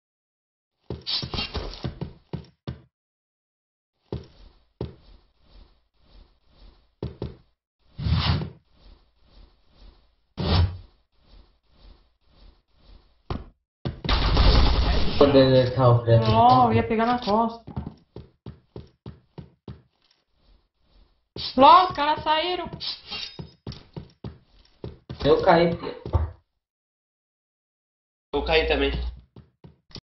Marcelo_02
Os seguintes videos mostram a verdade ... o Marcelo me chama de Aimbot pois diz que eu varo coisas e que só dou HS. assistam e tirem as suas conclusões.. ahh e infelizmente Marcelo tem que continuar treinando para poder entrar na line de Arma